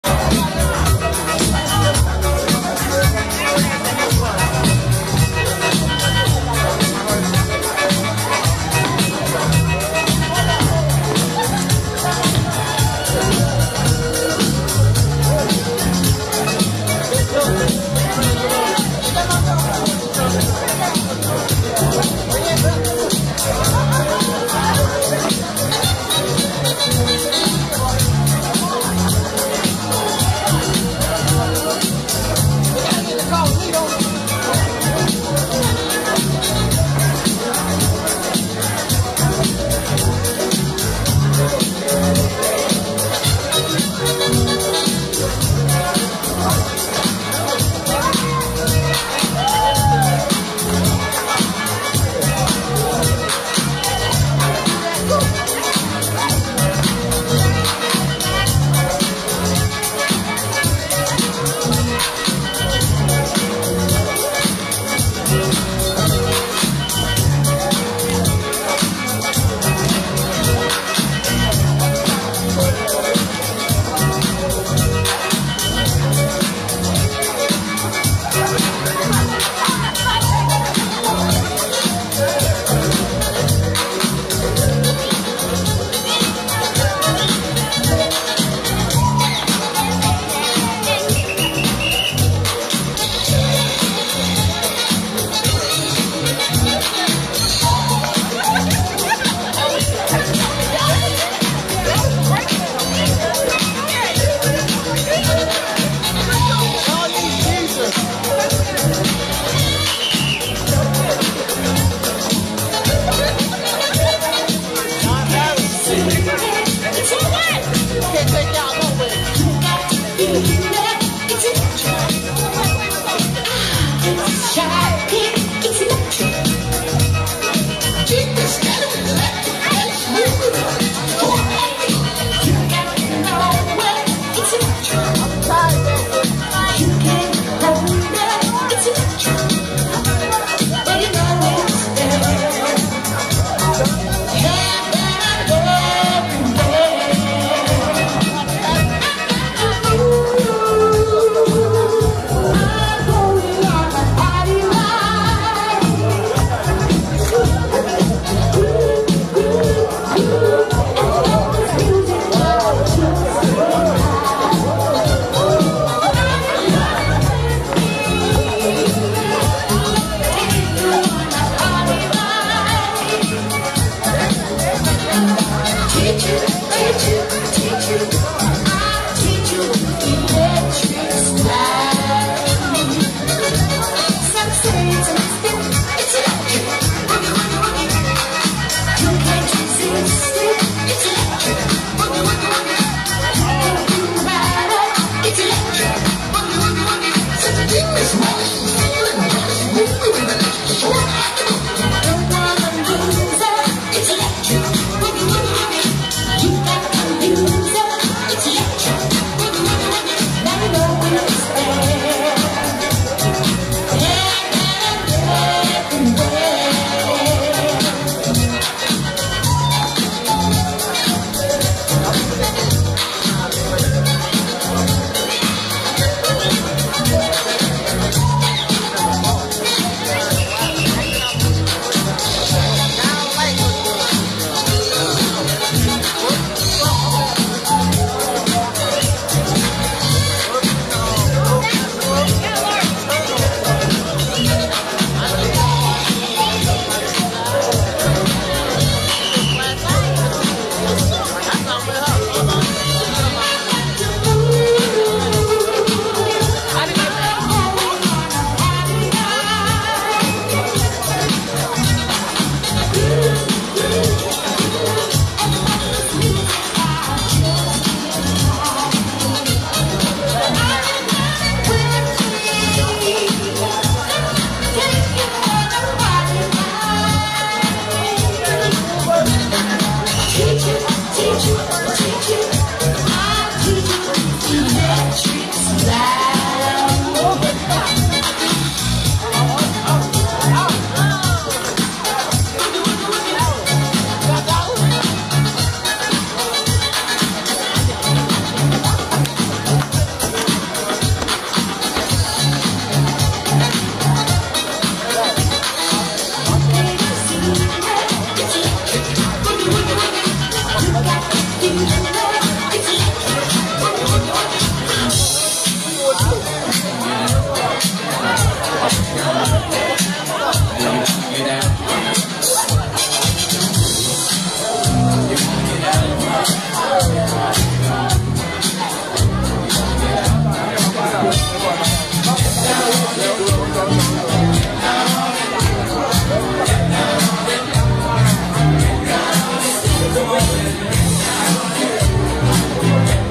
Nigeria/Jamaica Wedding/ Electric Slide Dance! USA
Nigerian Man weds Jamaican Lady in New Ark, New Jersey USA. Nigerian Ayorinde  and Krystal brought out the best in the African- Caribbean culture and a mix of the sha sha dance! 17th April 2009.